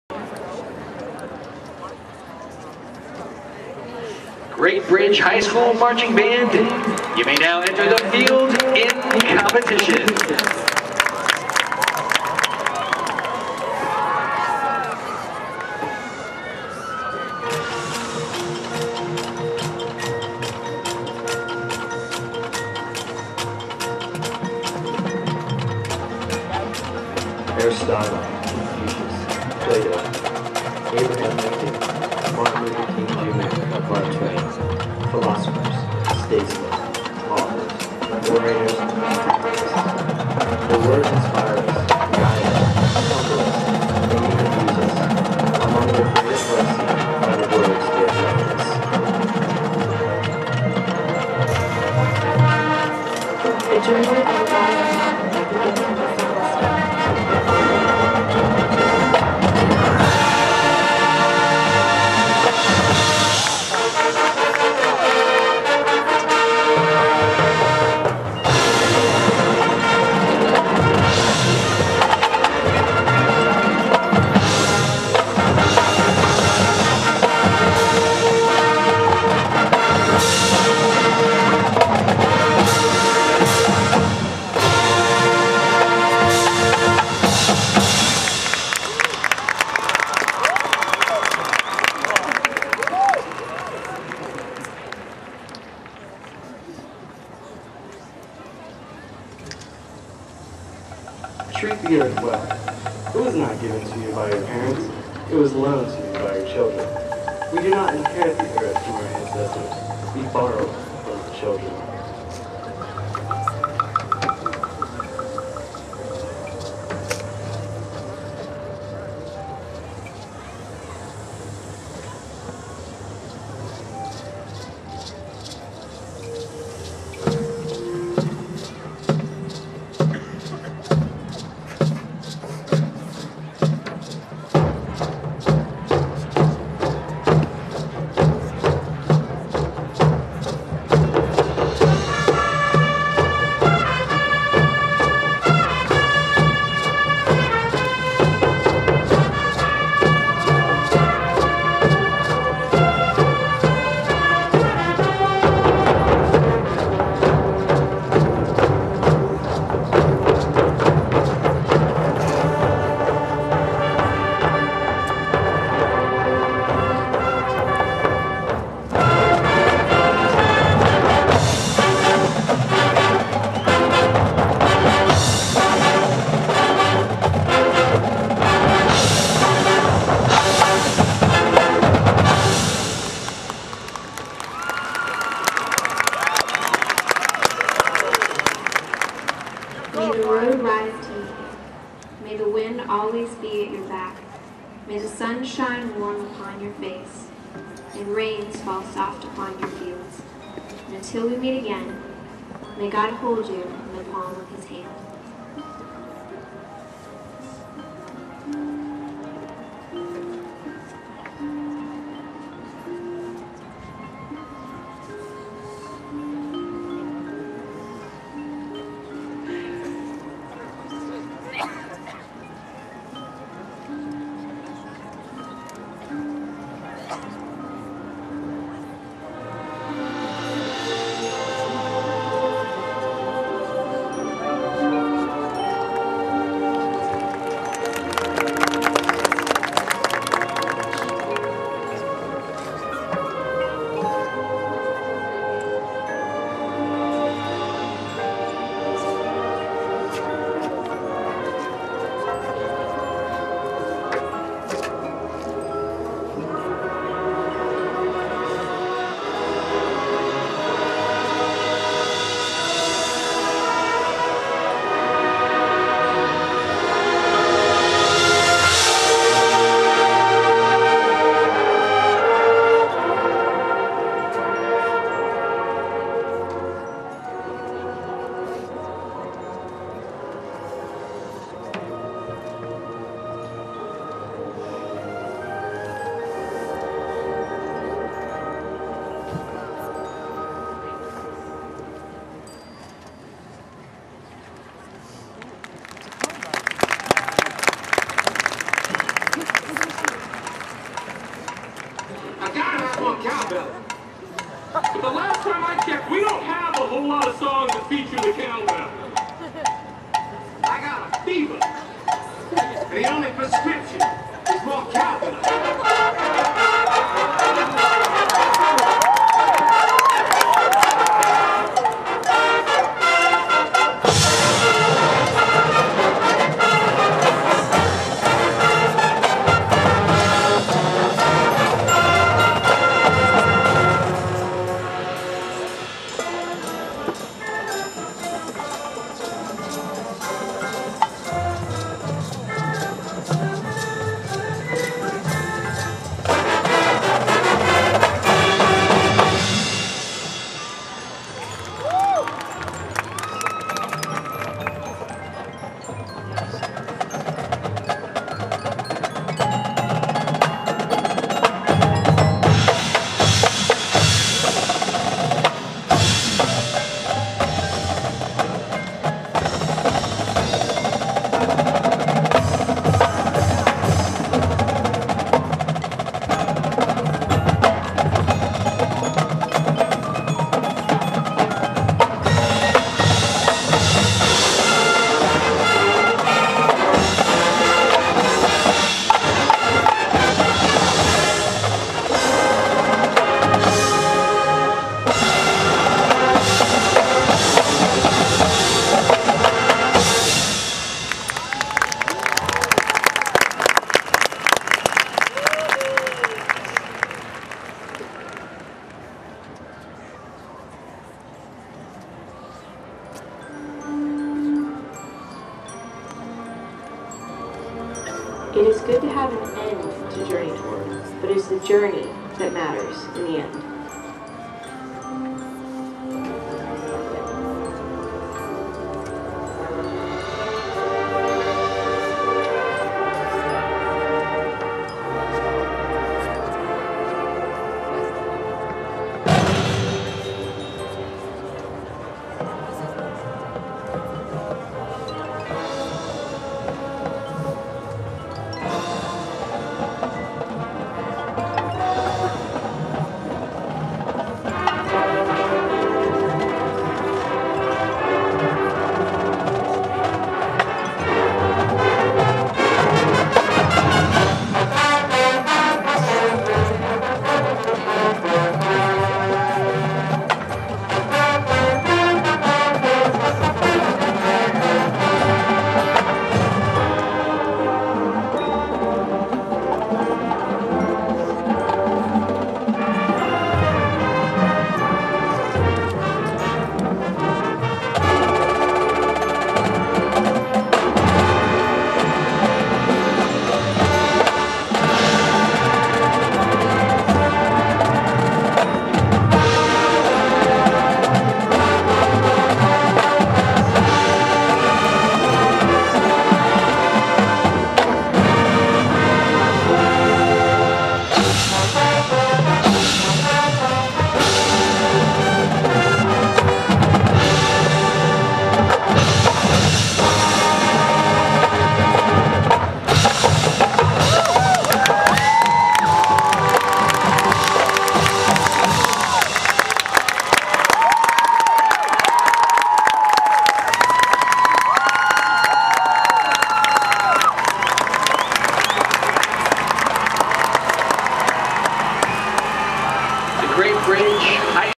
Great Bridge Marching Wildcats- JMU Parade of Champions 2007
Great Bridge Marching Wildcat's performance at the 2007 James Madison University Parade of Champions.